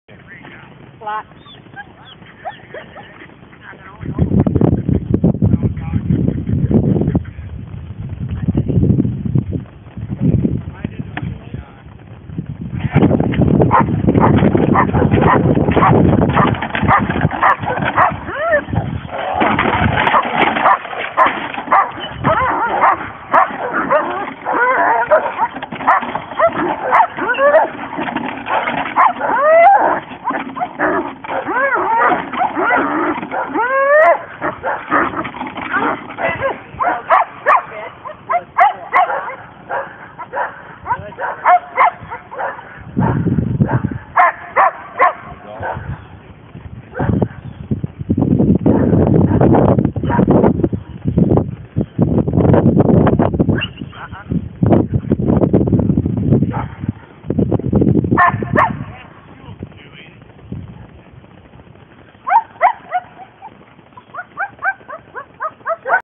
K-9 Perfection and Betty doing Personal Protection training
John L Lockeet Owner/ MT Trainer of K9-Perfection teaching Betty Mathena's GSD Bogie Personal Protection "The Attack on Handler"